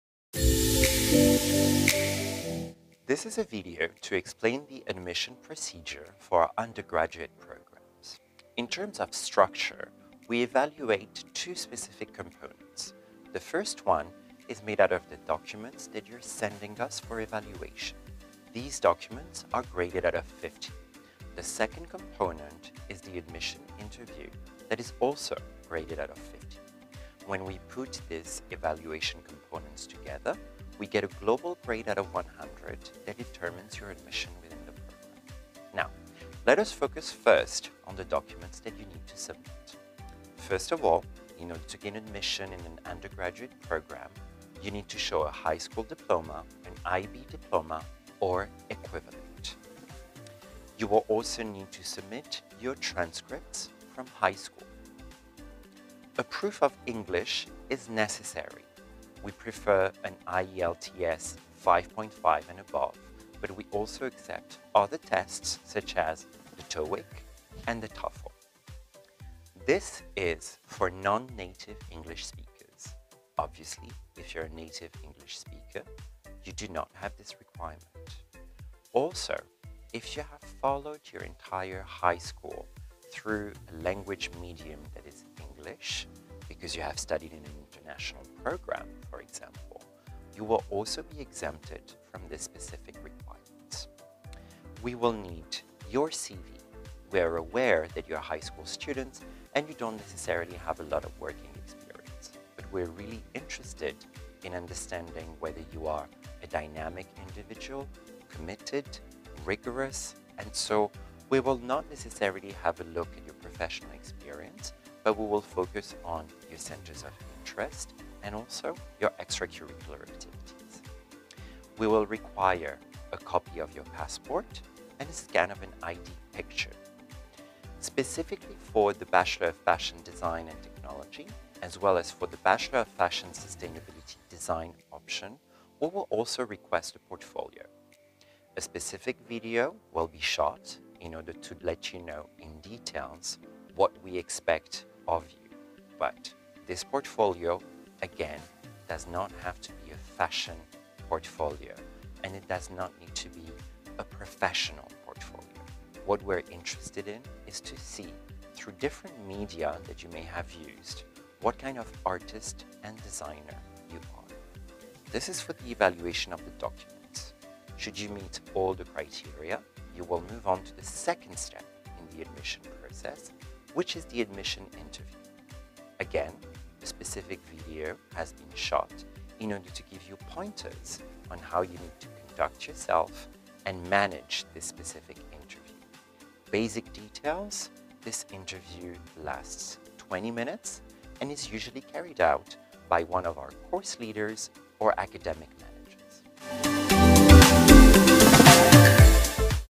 Undergraduate Admissions Requirements at IFA Paris - CEO Jean-Baptiste Andreani
CEO Jean-Baptiste Andreani breaks down admissions requirements for our Undergraduate Bachelor programs at IFA Paris. Listen carefully and learn more now!